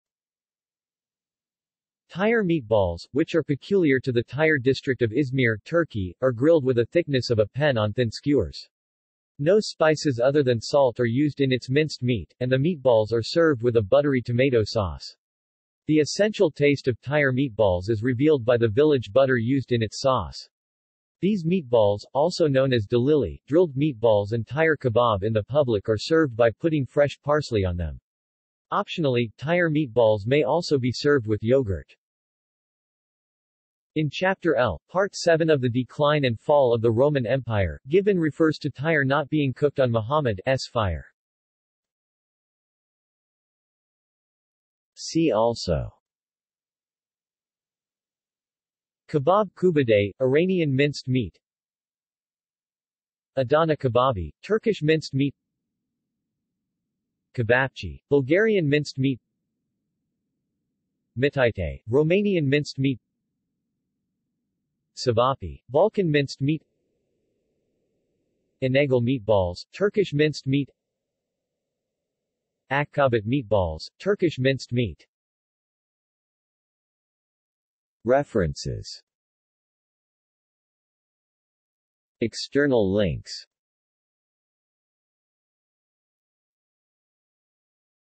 Tire meatballs | Wikipedia audio article
This is an audio version of the Wikipedia Article:
Tire meatballs

SUMMARY
Tire meatballs, which are peculiar to the Tire district of İzmir, Turkey, are grilled with a thickness of a pen on thin skewers. No spices other than salt are used in its minced meat; and the meatballs are served with a buttery tomato sauce. The essential taste of Tire meatballs is revealed by the village butter used in its sauce. These meatballs, also known as Delikli (drilled) meatballs and Tire Kebab in the public are served by putting fresh parsley on them. Optionally, Tire meatballs may also be served with yoghurt.
In chapter L, part VII of The Decline and Fall of the Roman Empire, Gibbon refers to tire not being cooked on Mohammed' s fire.